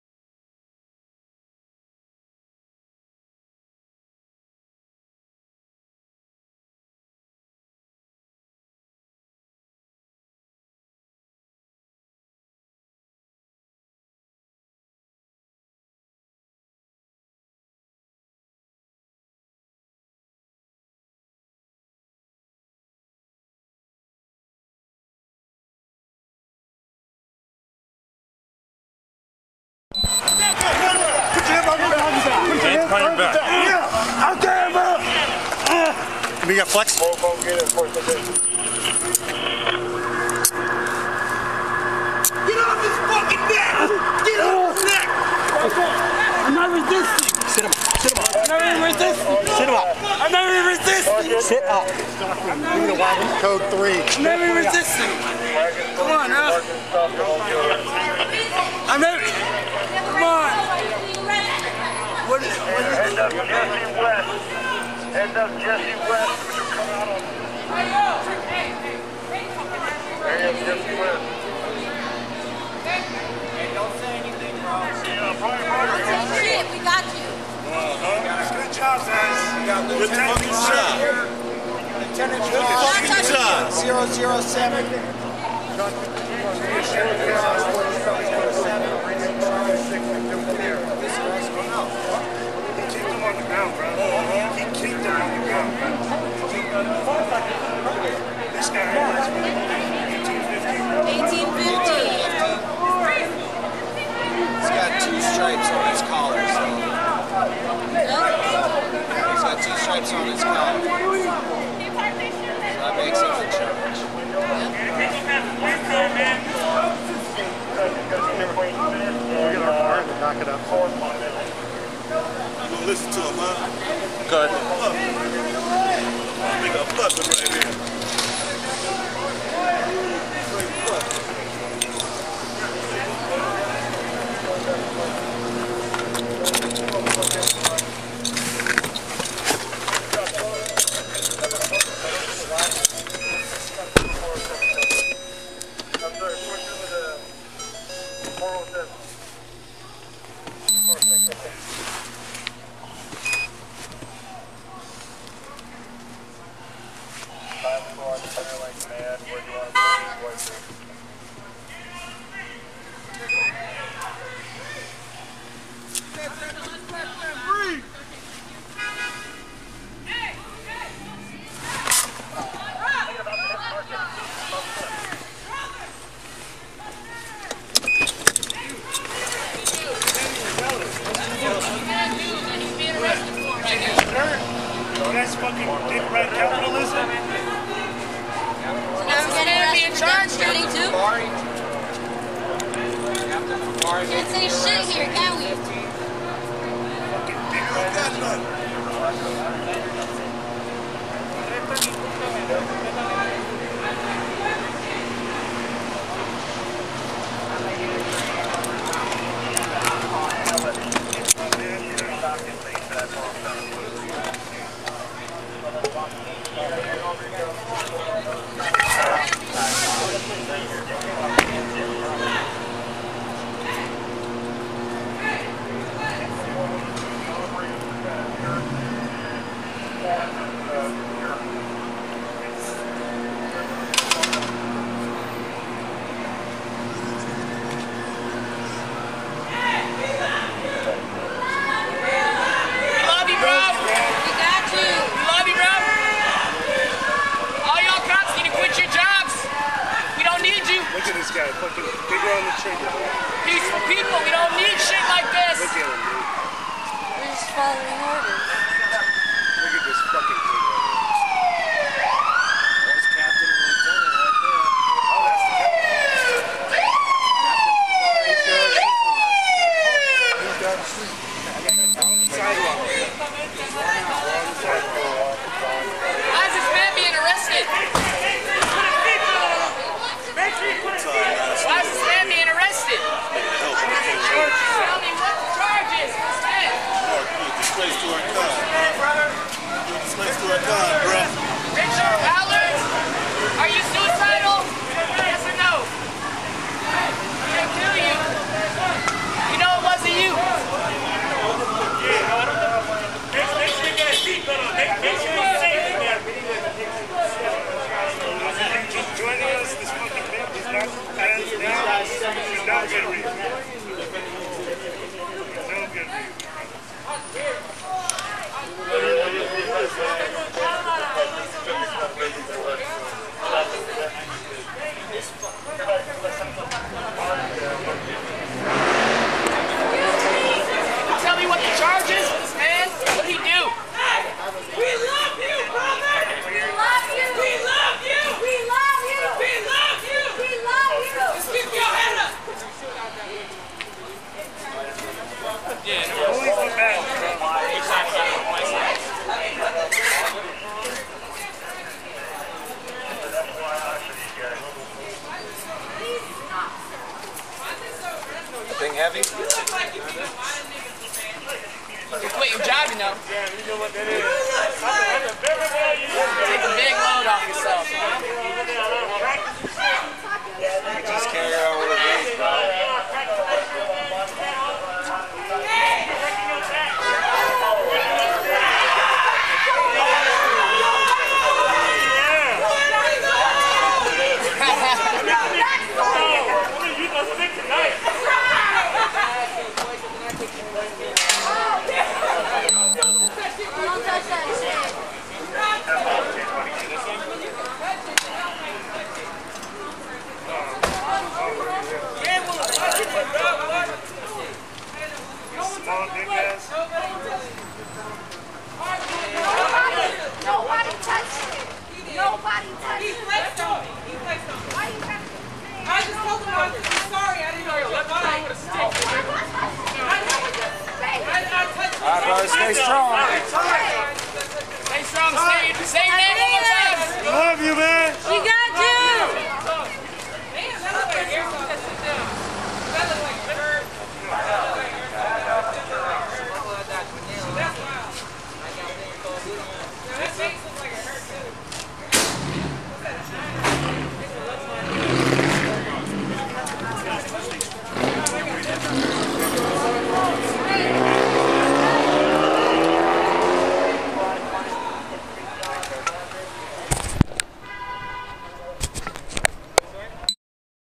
POV 3, Alleged Aggravated Assault on Police 200327462 - SFPD Bodycam - 2020 George Floyd Protests
Coded by SFPD as "Assault, Aggravated, On Police Off., W/ Other Weapon"

Unofficial copy of a public record released by SFPD under the California Public Records Act and San Francisco Sunshine Ordinance pursuant to a request for all uses of force by SFPD and footage of alleged crimes during the George Floyd protests in 2020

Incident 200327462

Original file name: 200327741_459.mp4

for consistently successful freedom of information advocacy.